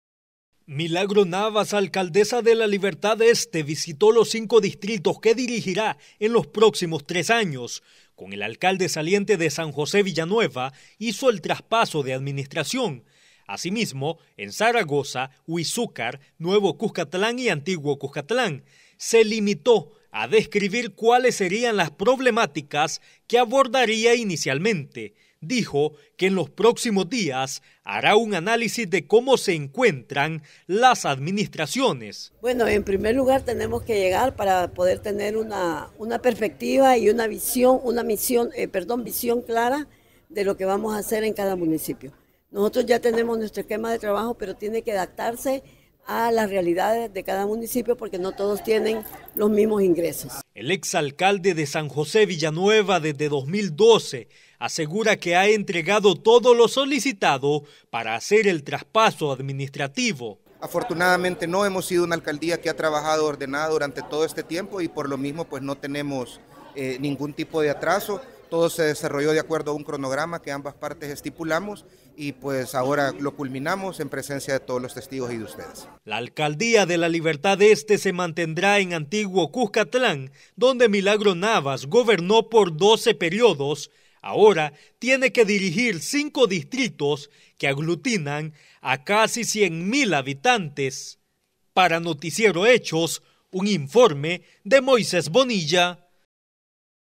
Traspaso de mando en La Libertad Este
La alcaldesa de La Libertad Este, Milagro Navas, recibió la administración de los 5 distritos de ese municipio.